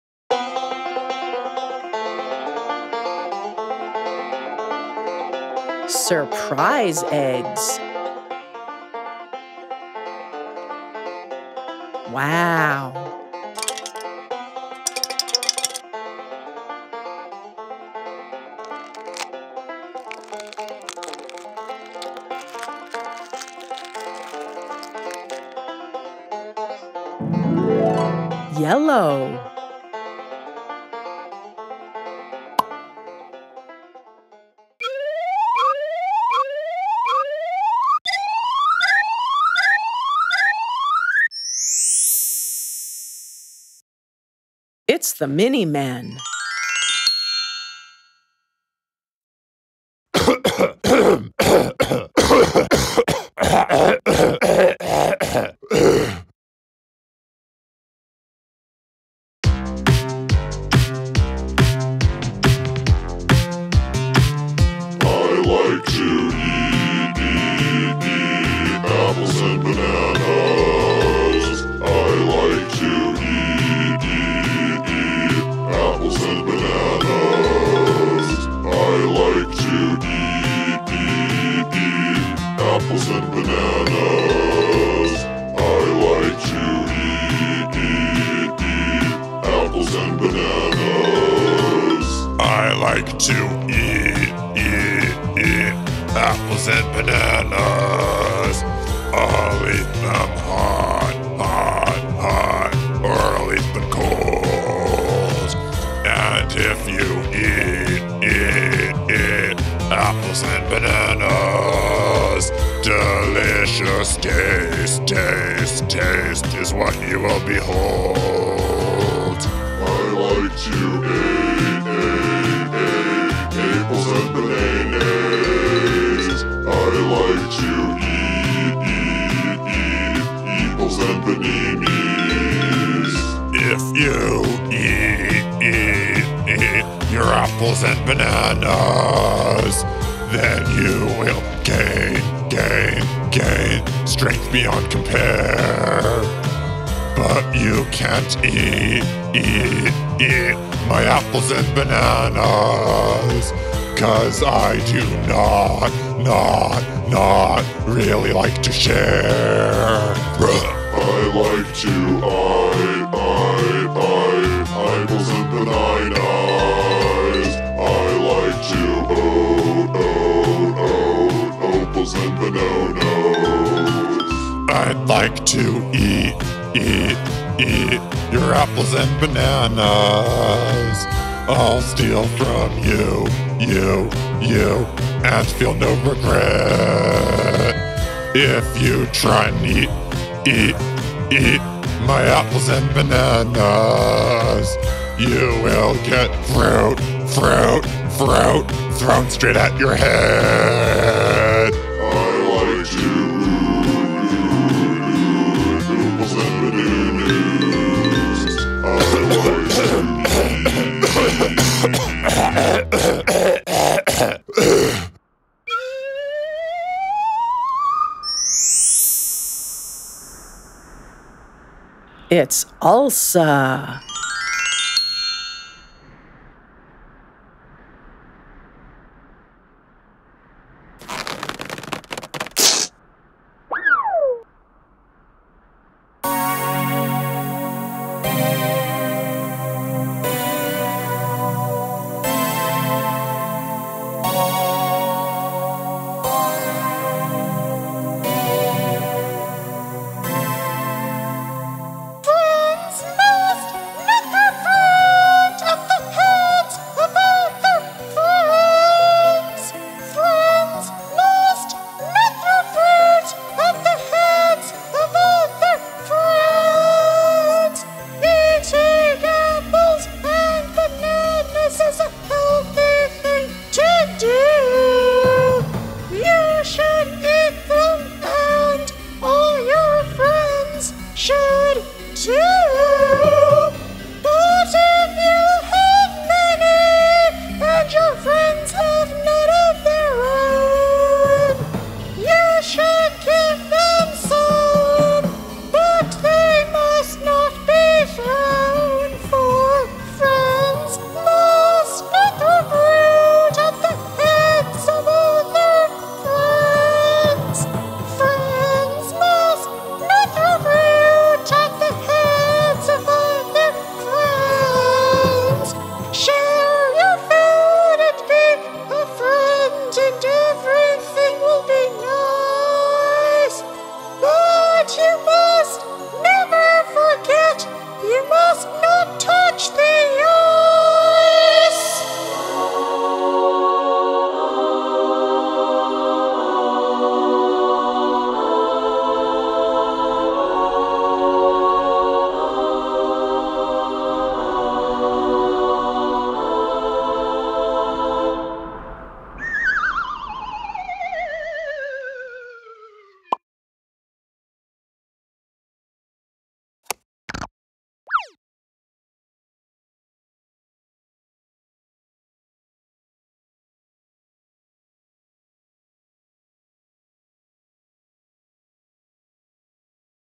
Surprise Eggs 2: Banana Yellow Egg - EdukayFUN
The next installment in the Surprise Eggs saga. What could be inside the Yellow Egg?